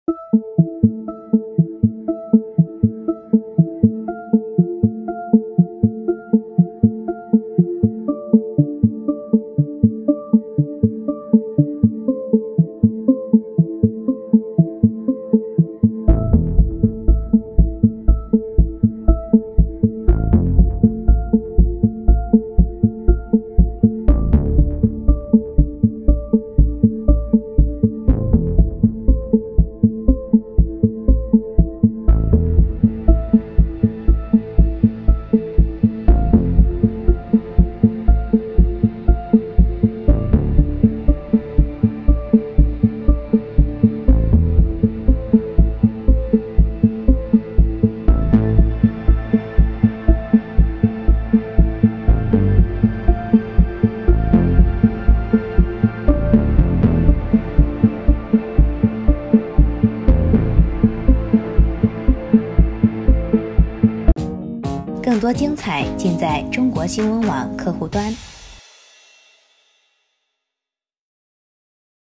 香港示威者机场静坐阻碍通行与旅客发生冲突
8月13日下午2时许，在香港国际机场，示威者到离境大堂外抗议及静坐，3时后，示威者已坐满离境大堂。一名身型稍胖的女士在示威者间穿行，需要机场职员协助接过行李，并要职员伸手扶着越过示威人群，而静坐的示威者不肯让路。当女士越过示威者时似乎不慎与其发生碰撞，示威者随即发怒。经机场职员劝说后，情况大致平静，只是旅客仍要跨过示威者才能通行。